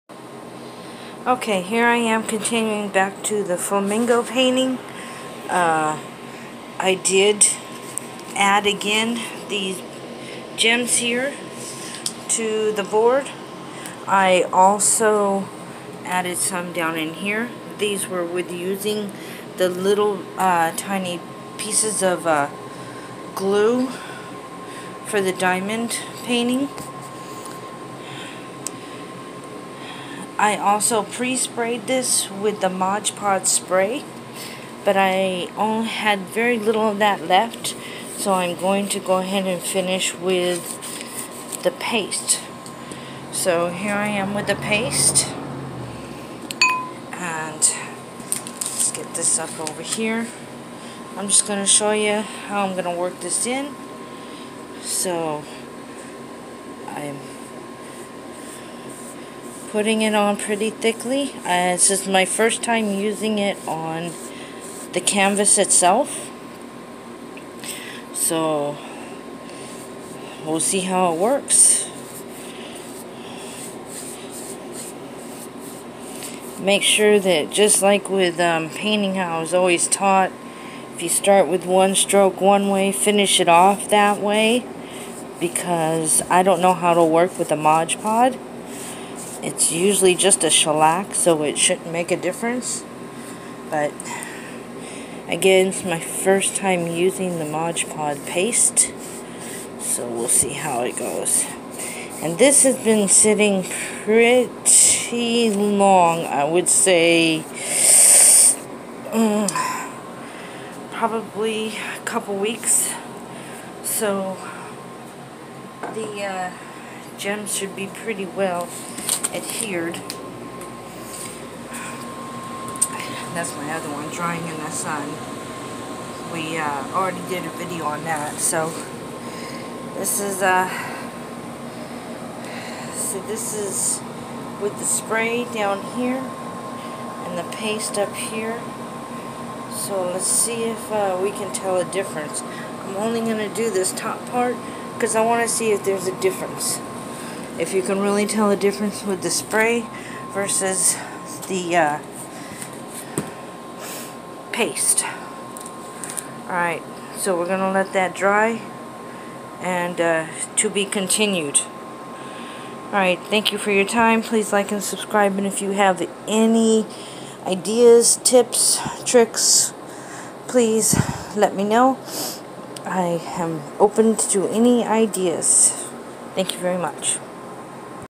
Flamingo Board 2 of
This Flamingo Diamond painting is me finishing at all with the Mirage pod paste the gloss luxury finish it's my first time using it so I'm seeing how it will work out. Please note in the video I do keep referring to the Mod Podge as a paste and not a sealer. Thank-you for your understanding in this error.